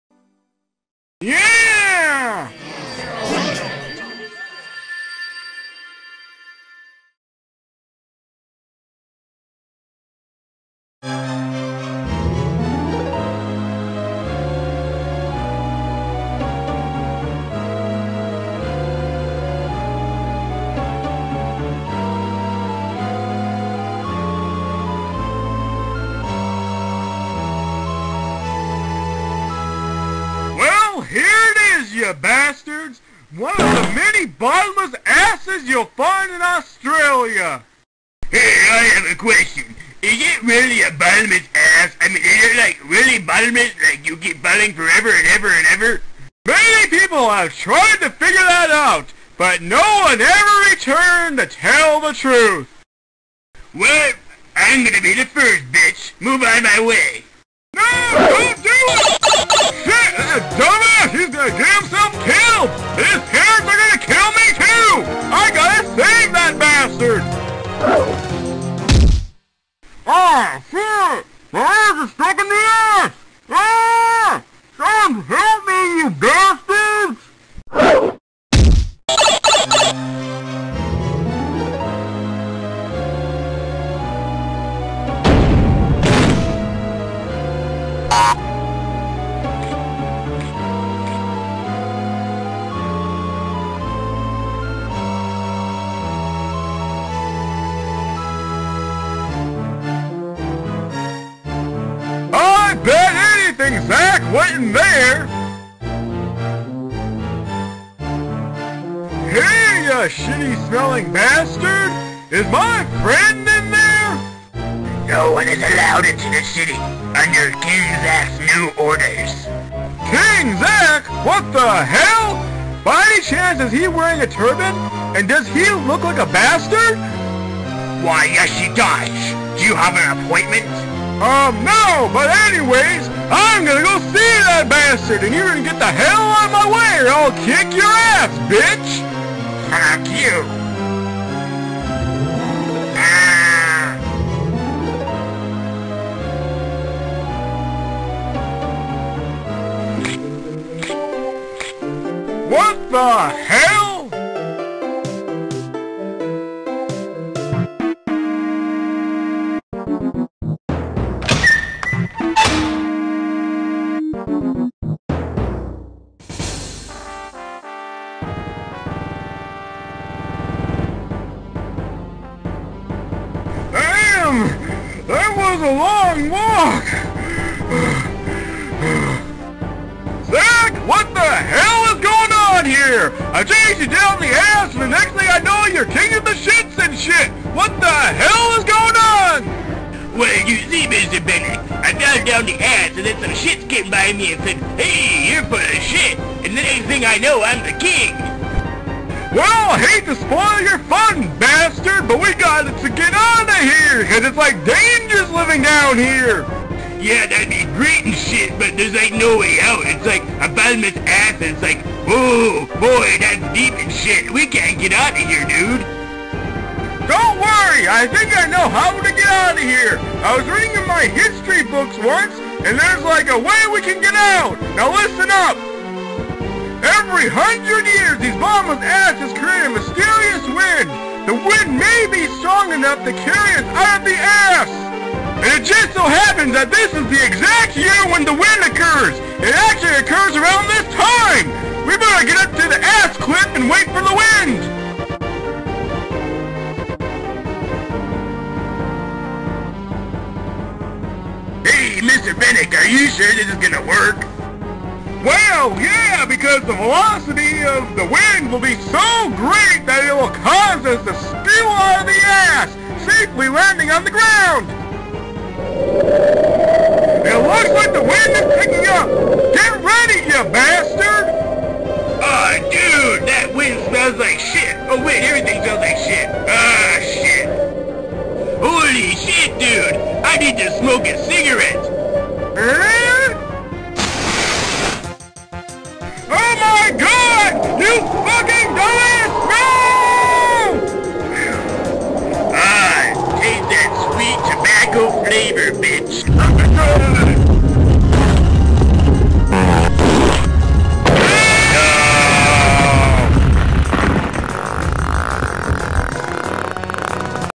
Mr. Binek’s Class 3DMM - Episode 25: Holes No Man Has Touched
Zakk is crowned the King of the Shits in the Bottomless Ass of Australia.

Film History:

Taken from the Lilredhead Studios website (written on July 21st, 2007):

All righty, for some reason, this idea came up all the way back in Episode 9. In just a random sentence I blurted out, Mr. Binek mentions "the bottomless asses in Australia". And a few months later I made an episode about said asses. This episode was a favorite of mine for quite some time due to how bizarre it was.

Film Information:

Directed by: Chad Troftgruben
Written by: Chad Troftgruben
Edited by: Chad Troftgruben
Starring: Chad Troftgruben
Released: September 18th, 2001
Runtime: 5 Minutes, 1 Second
Type: 3DMM
Genre: Comedy